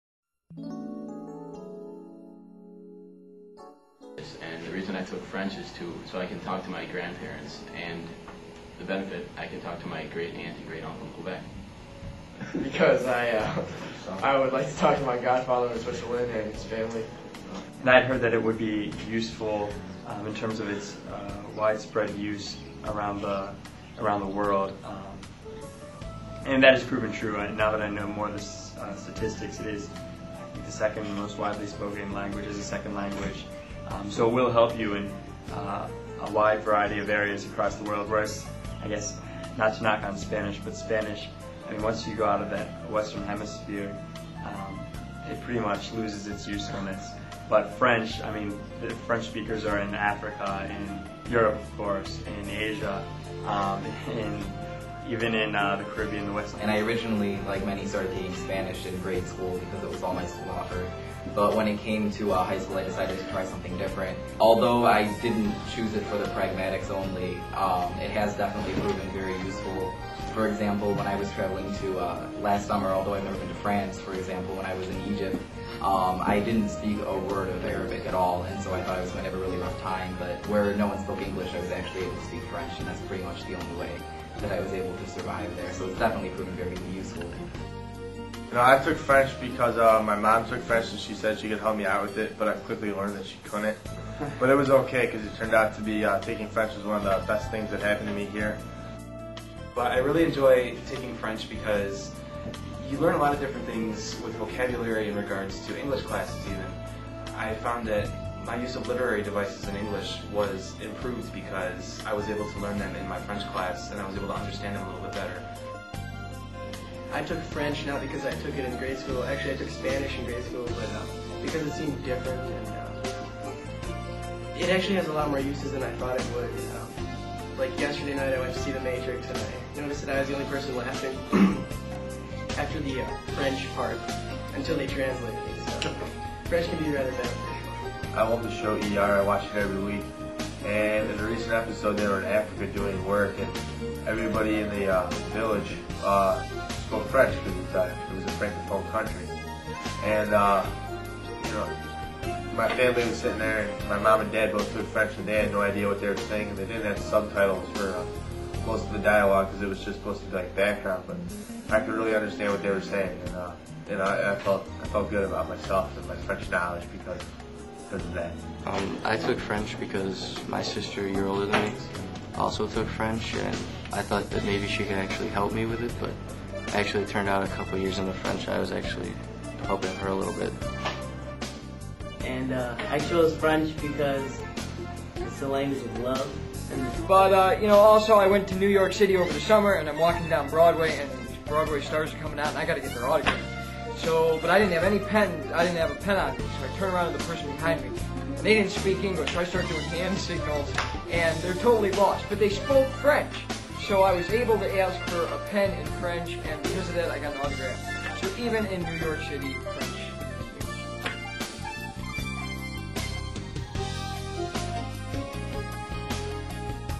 Why Study French
Saint Ignatius students explain why they chose to study French and discuss the benefits of their choice.